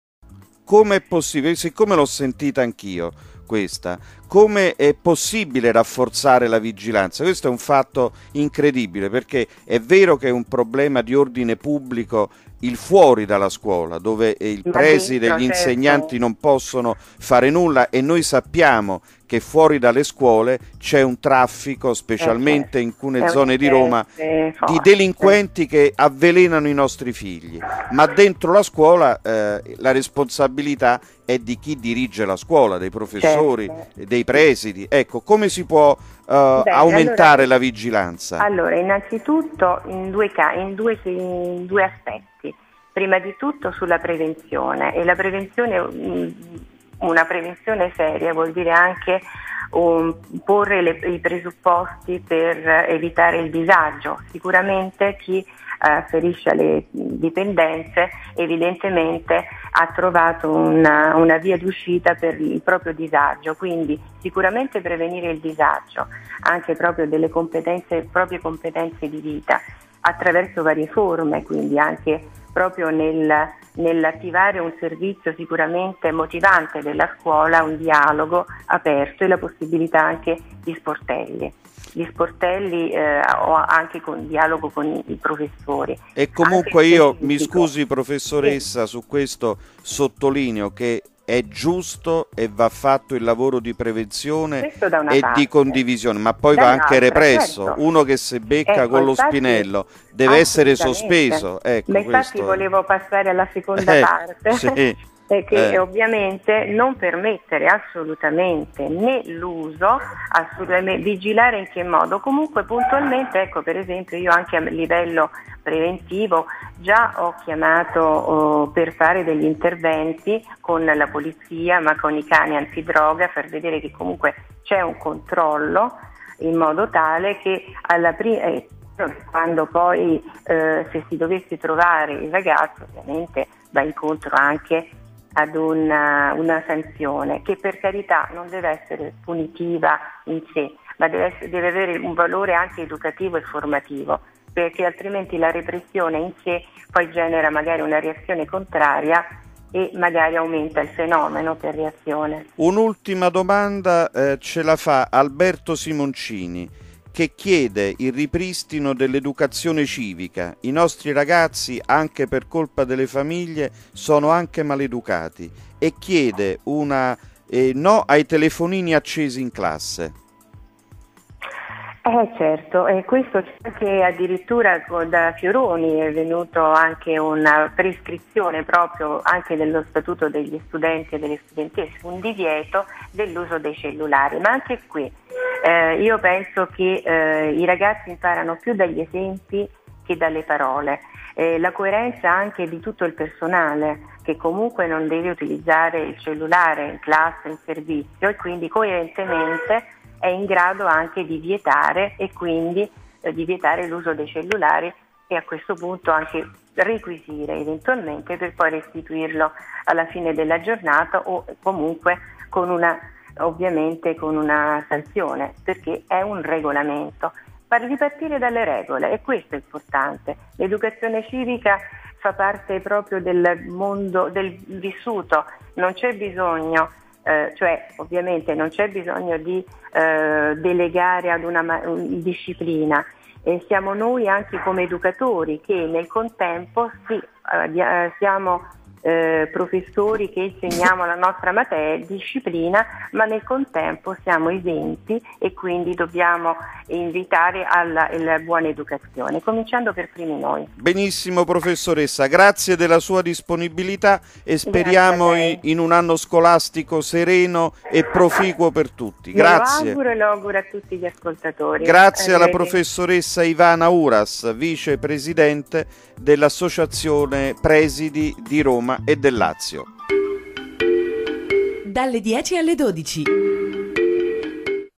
IVANA URAS PARTE 2 (VICEPRES. DEI PRESIDI DI ROMA E DEL LAZIO) PRIMO GIORNO DI SCUOLA
FRANCESCO GESUALDI INTERVISTA IVANA URAS (VICEPRES. DEI PRESIDI DI ROMA E DEL LAZIO).